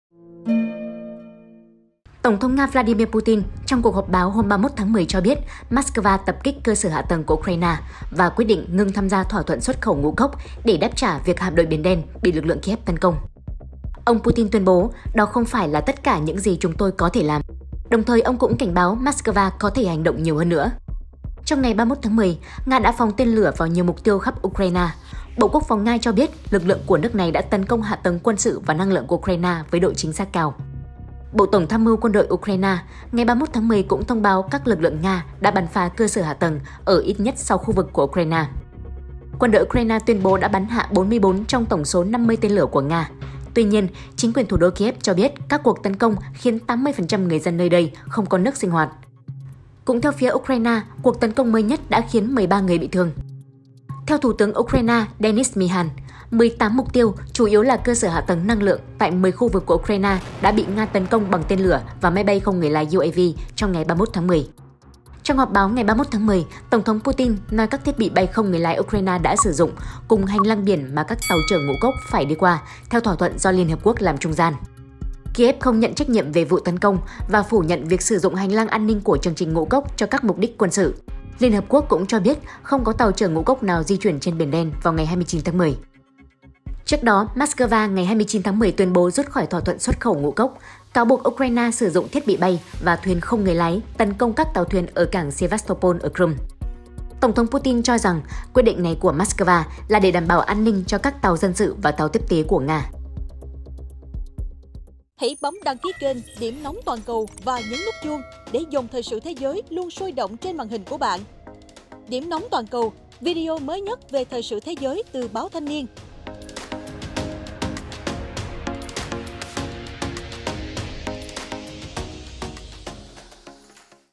Tổng thống Putin xác nhận đáp trả Ukraine vì Hạm đội biển Đen bị tấn công
Tổng thống Nga Vladimir Putin trong cuộc họp báo hôm 31.10 cho biết Moscow tập kích cơ sở hạ tầng của Ukraine và quyết định ngưng tham gia thỏa thuận xuất khẩu ngũ cốc để đáp trả việc Hạm đội biển Đen bị lực lượng Kyiv tấn công.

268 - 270 Nguyễn Đình Chiểu, Phường Võ Thị Sáu, quận 3, TP HCM